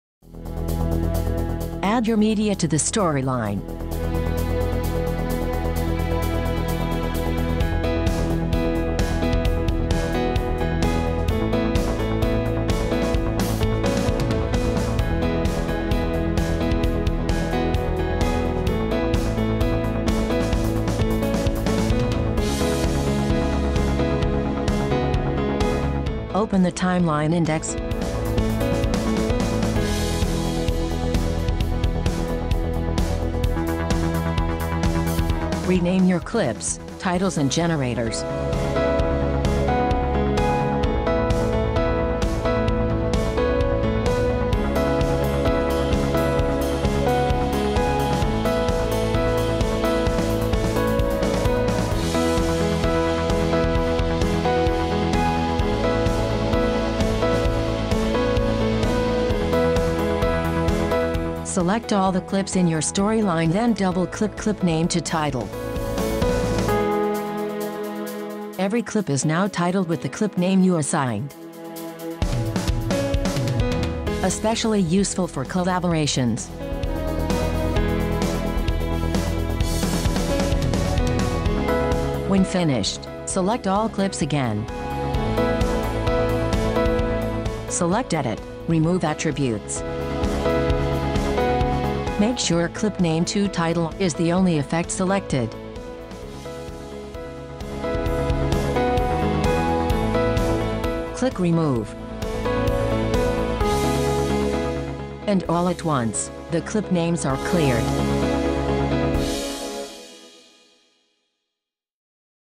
Clip Name to Title — as a collaboration tool
Clip Name to Title: a short demonstration of using this effect to quickly label every clip, including titles and generators, in your Final Cut Pro X storyline and when finished with the project, just as quickly remove the effect.

Clip Name to Title can be refashioned in almost any way you want, saved as an Effects Preset and applied as your customized version.  Customizations include: Font, Size, Alignment, Tracking, Font Color/Opacity, Background Color/Opacity and Height, Border Color/Opacity and Width. Position, Background Width (as a separate control) and Background Offset are also provided. All parameters except the Font and Alignment can be keyframed.  Keyframing is maintained in Effects Presets! There is also an OnScreen Control for easy positioning of the entire text/background effect.